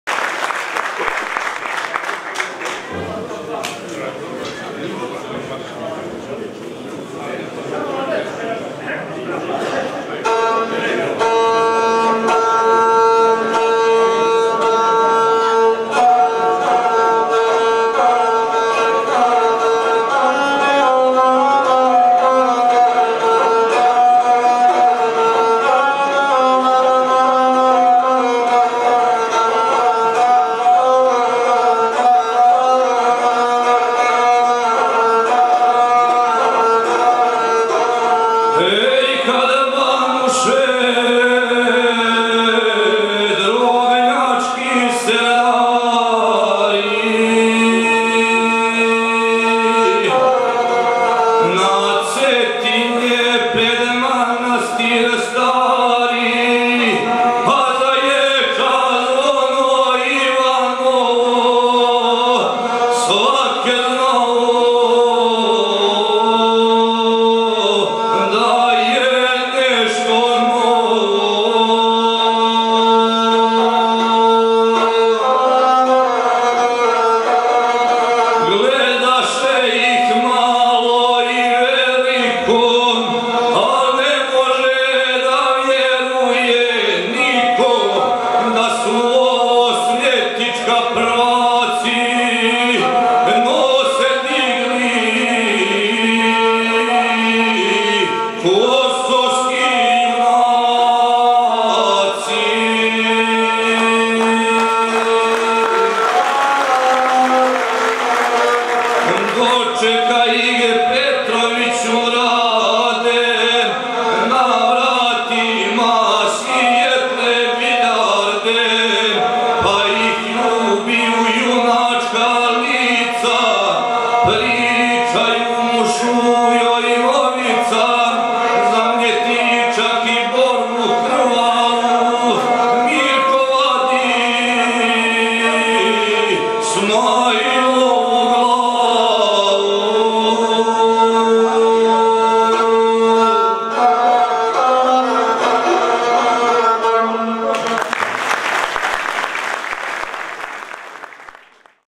Миломир Елез ГД Тешан Подруговић Гацко
Вече гусала и изворног пјевања, Билећа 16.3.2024.
Организатор ГД "Војвода Глигор Милићевић"